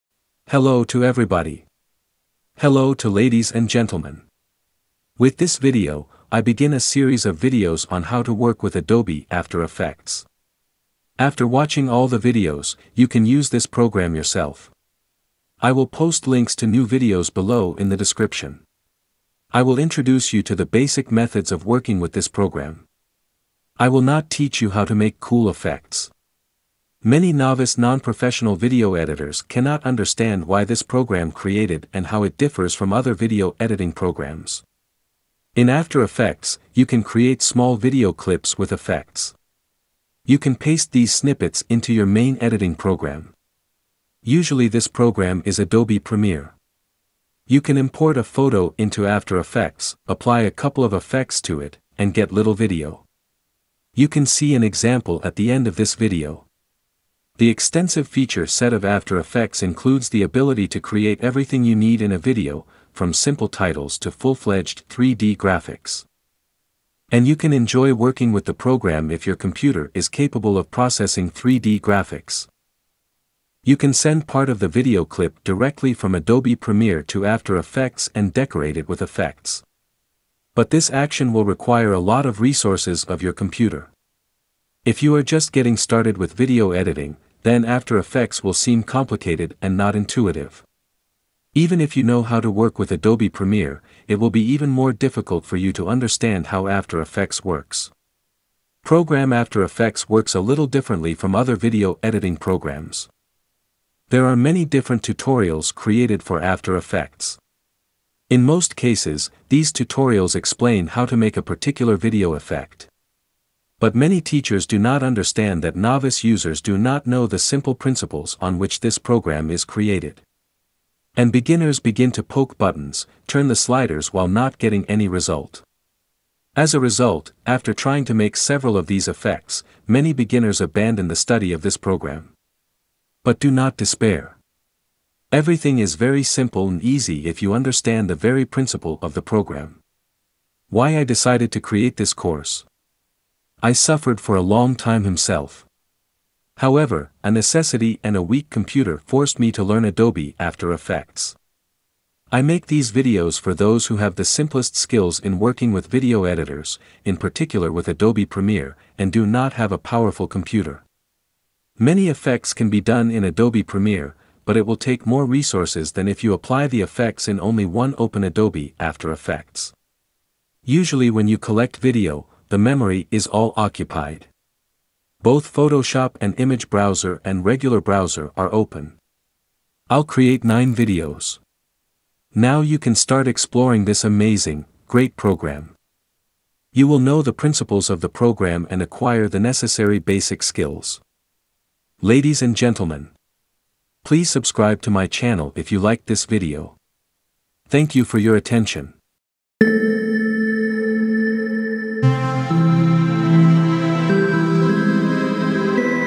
General Review Adobe After Effects.
(multilingual subtitles) With this video, I begin a series of videos on how to work with Adobe After Effects.

I will introduce you to the basic methods of working with this program. I will not teach you how to make cool effects. Many novice non-professional video editors cannot understand why this program created and how it differs from other video editing programs.
In After Effects, you can create small video clips with effects. You can paste these snippets into your main editing program. Usually this program is Adobe Premiere. You can import a photo into After Effects, apply a couple of effects to it, and get little video. You can see an example at the end of this video.